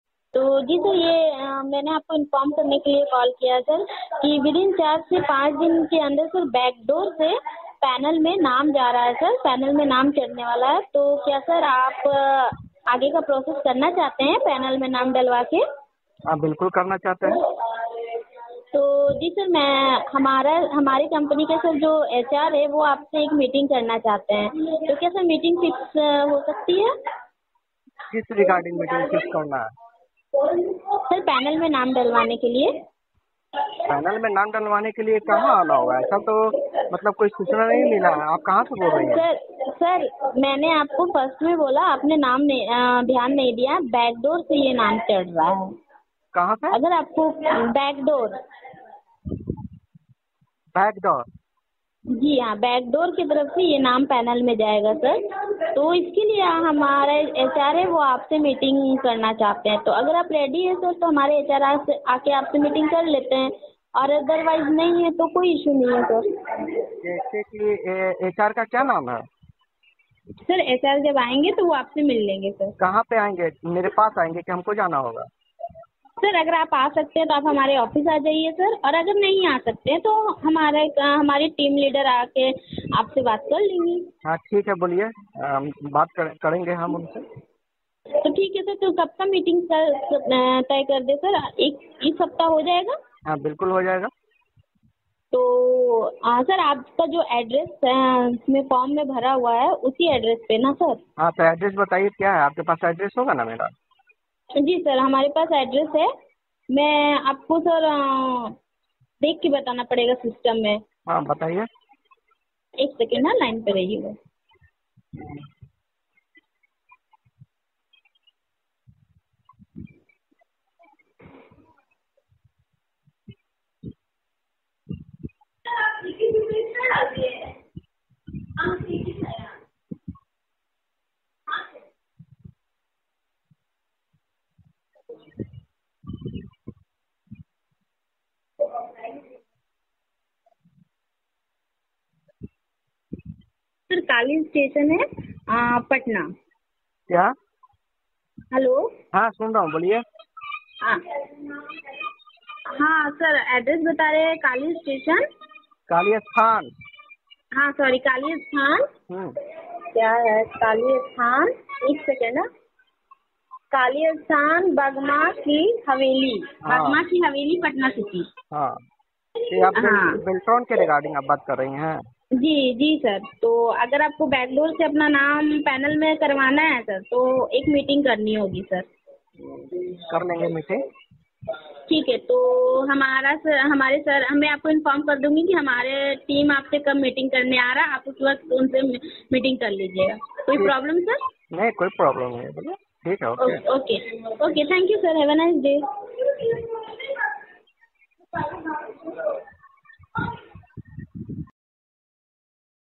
LRC Joining me dhandhali pta nahi kya hoga ab , call recording vairal/
LRC Joining me dhandhali ka call recording vairal,,
Beltron deo

bihar gov
technical helping keshav